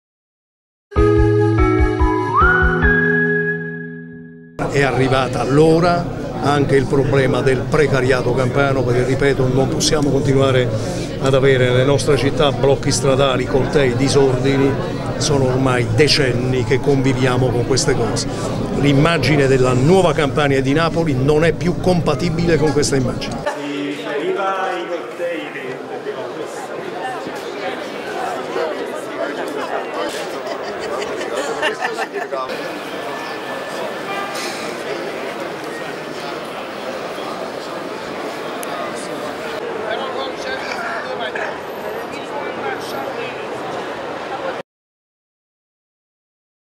De Luca: "Basta cortei", ma i disoccupati assediano la Regione
Perentorio il presidente della Regione Campania, Vincenzo De Luca, che a margine della conferenza stampa di fine anno lancia una provocazione: "Bisogna attuare delle politiche assistenziali per un certo numero di precari in questa Regione, perché non possiamo più tollerare che Napoli venga bloccata da trent'anni dai cortei di disoccupati". Mentre scandisce queste parole, Palazzo Santa Lucia è proprio circondato dagli storici Precari Bros che protestano per il proprio posto di lavoro.